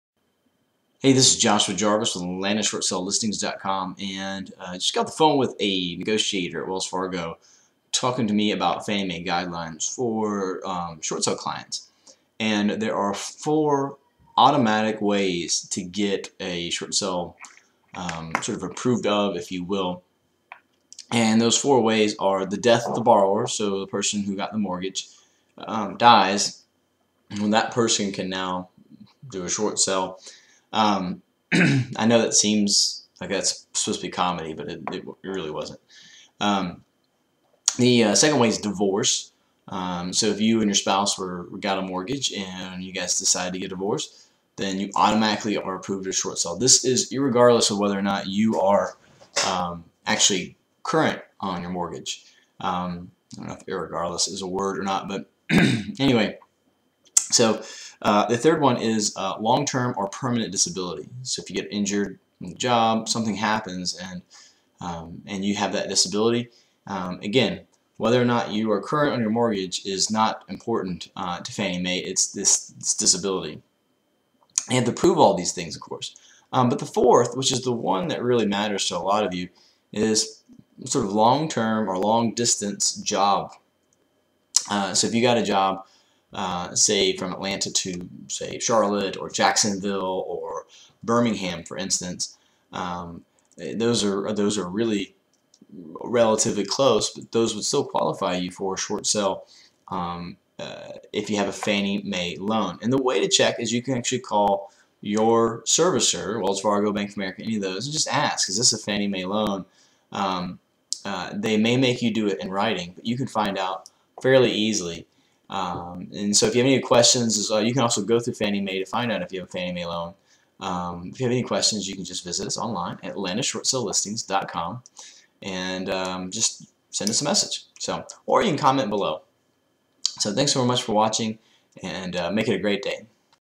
Fannie Mae 4 Ways To A Guaranteed Short Sale | Atlanta Short Sales
- There are 4 ways that are almost guaranteed to allow you to short sale and be current.  We talk about those in this video, if you want to remain current or just want to short sale and you have one of these 4 situations, then you'll have no problem as long as your loan is owned by Fannie Mae.